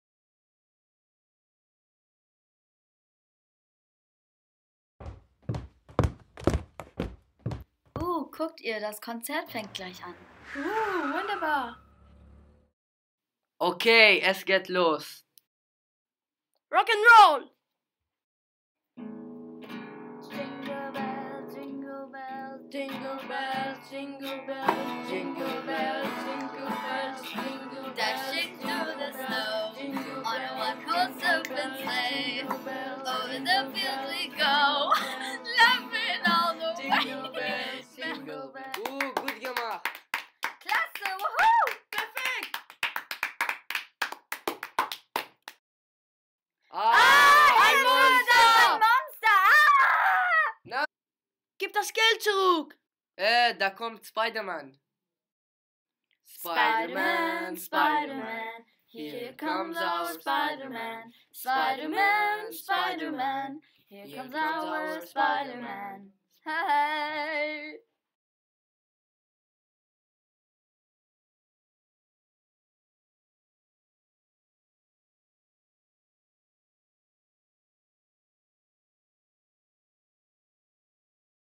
Heldenweihnacht
Kinder aus einem Vorkurs am Hermann-Böse-Gymnasium haben sich kleine Geschichten ausgedacht.